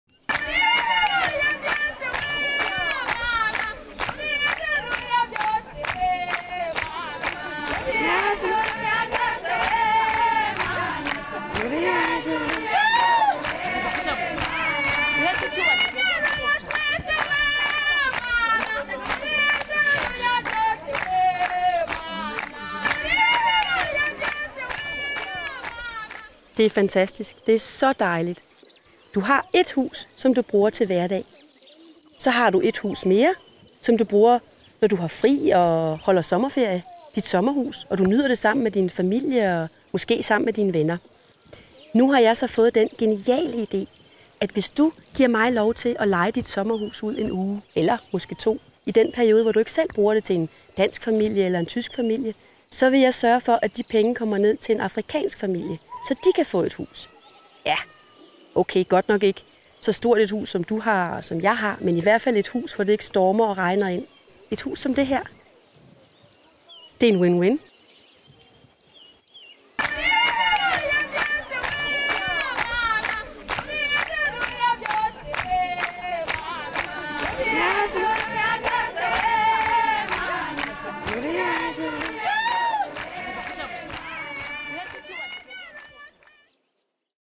Sommerhus for Afrika
Hvis du har et sommerhus
Konceptet er kort fortalt, at du gennem "Sommerferie.nu" kan udleje dit sommerhus og på en enkel og direkte måde støtte ADRA's arbejde med at give flygtninge tag over hovedet. Indtægten fra udlejning i én eller flere uger giver du til ADRA gennem projekt "Sommerhus for Afrika".